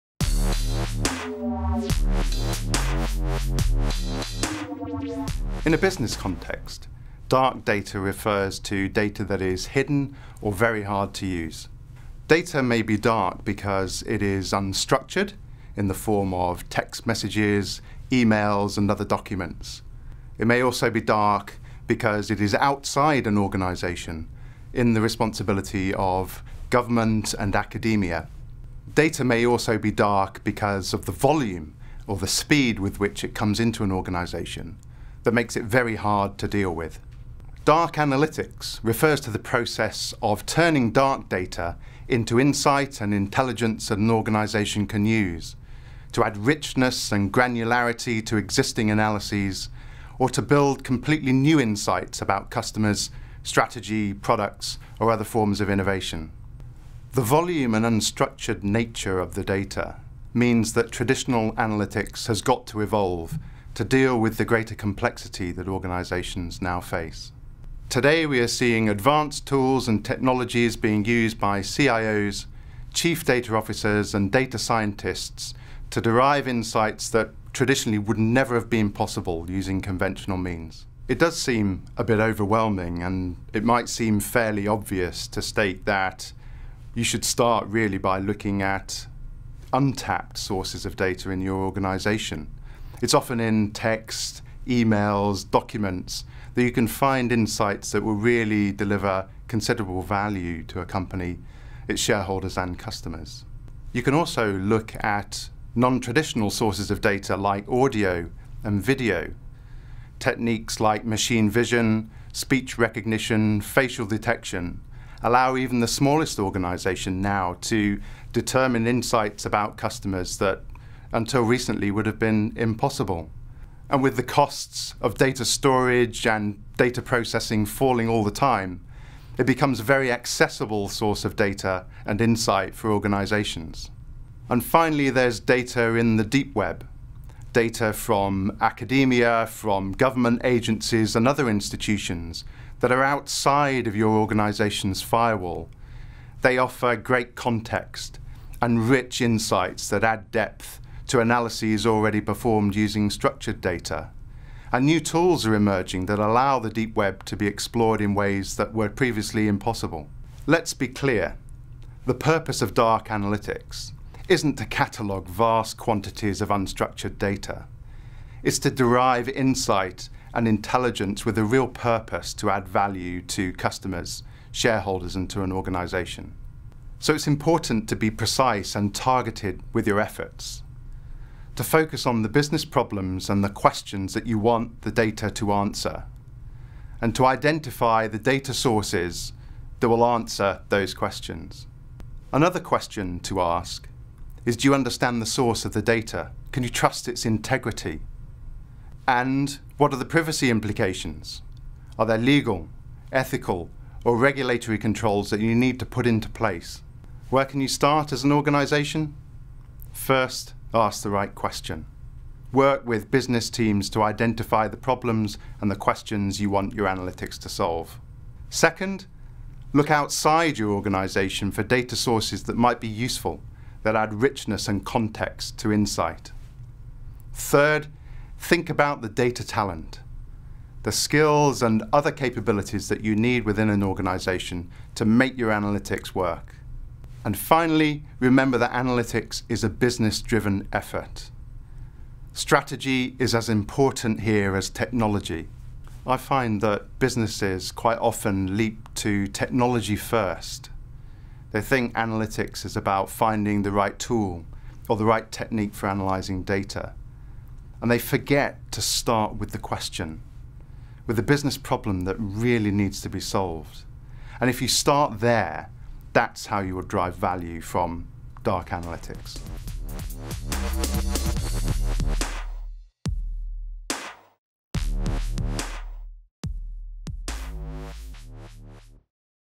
Deloitte UK: Tech Trends 2017 - Dark Analytics
Advances in computer vision and pattern recognition allow companies to unlock insights from unstructured data that until now, have been lost in the dark.